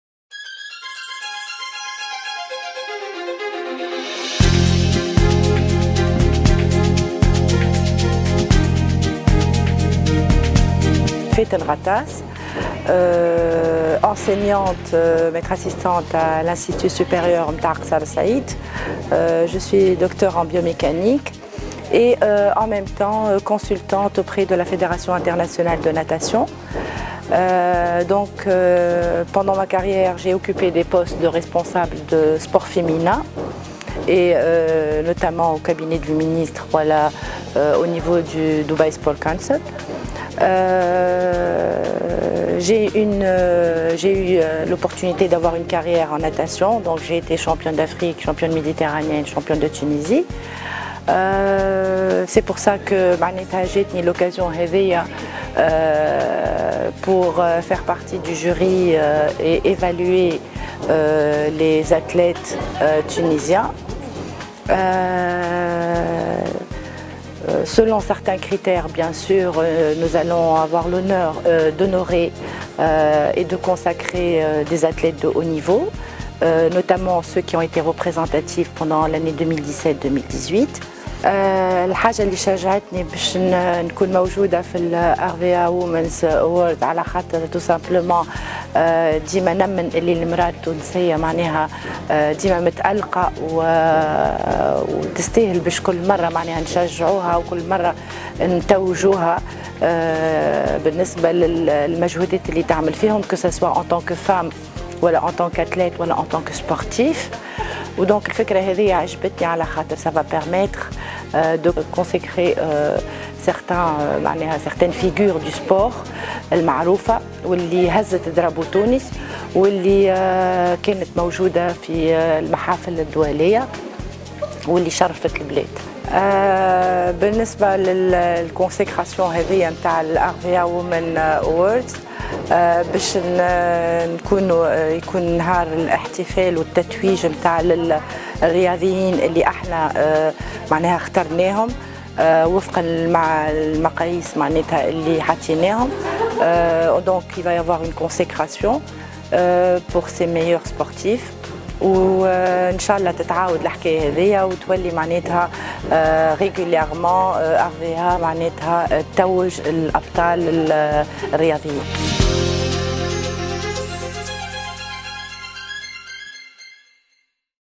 Faten Ghattas Membre du Jury ARVEAWOMENAWARDS.
Allocution de Mme Faten Ghattas Azzouz , Enseignante Universitaire à L'INEPS, Ex championne en Natation et Membre du Jury ARVEAWOMENAWARDS.